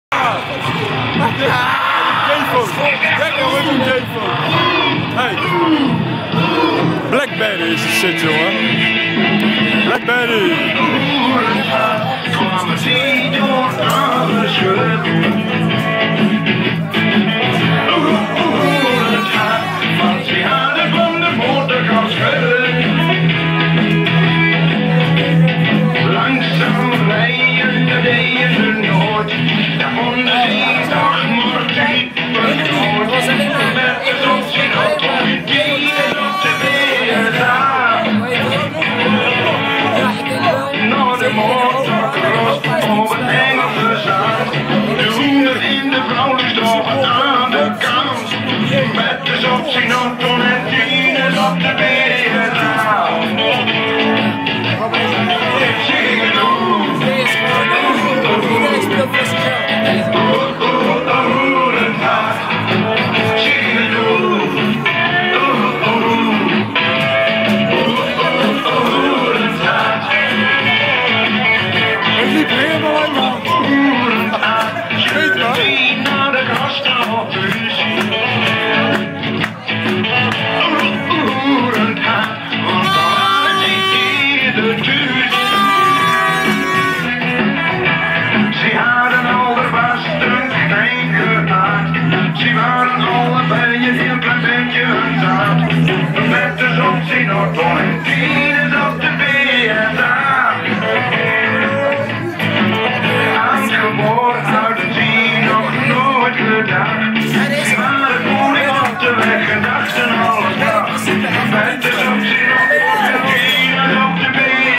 BV Rooi kampioen 2011
Bv rooi de beslissrnde wedstrijd tegen Deurne 1. Het dak gaat eraf...